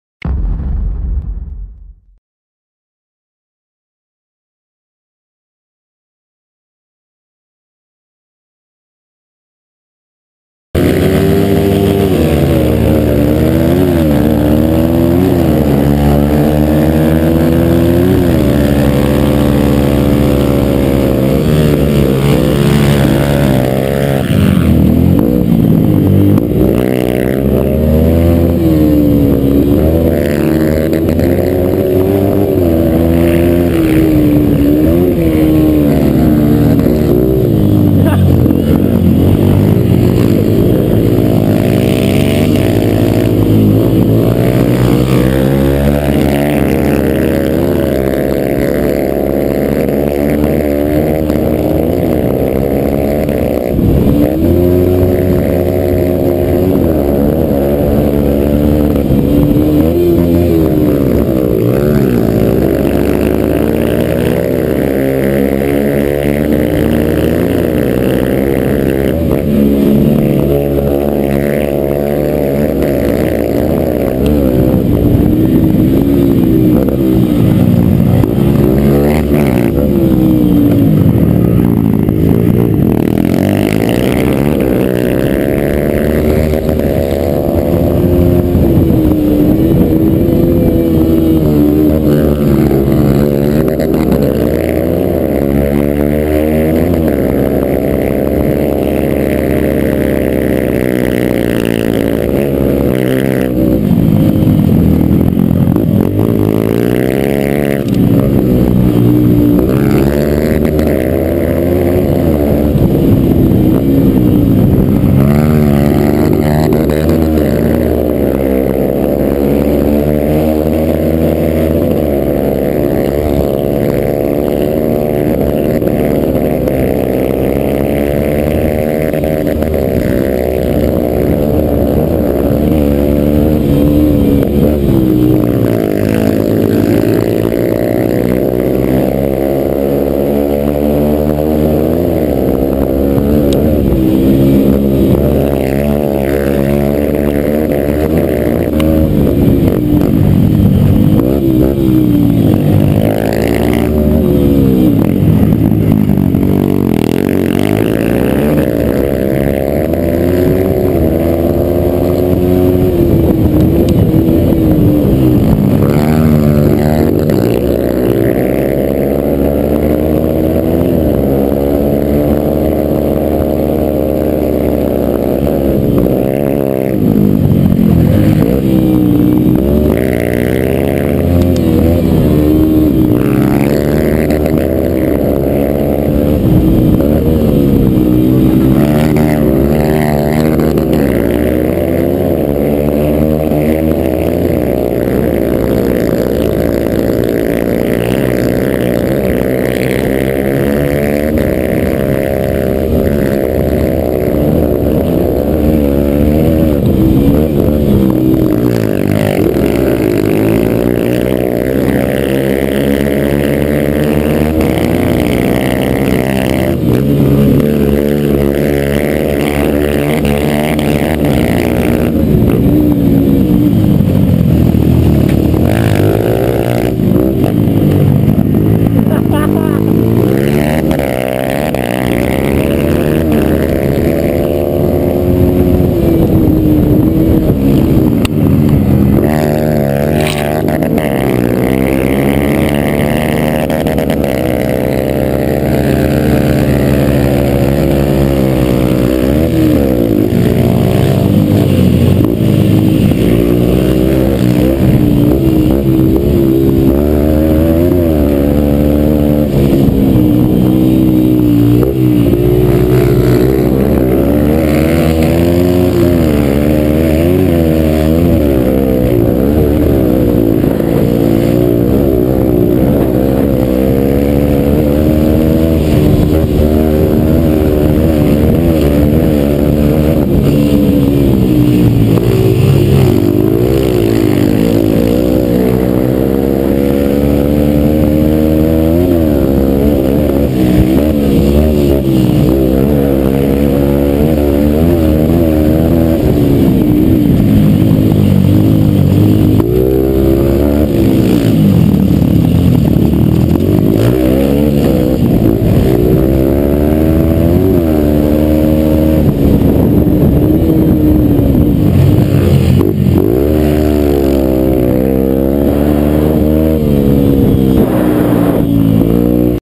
GoPro - Melhor Pega de Todos Os Tempos ! TTR-125 vs CG150
GoPro - Vini TTR-125 , Fabiano CG150 2012 ! Canelinha - SC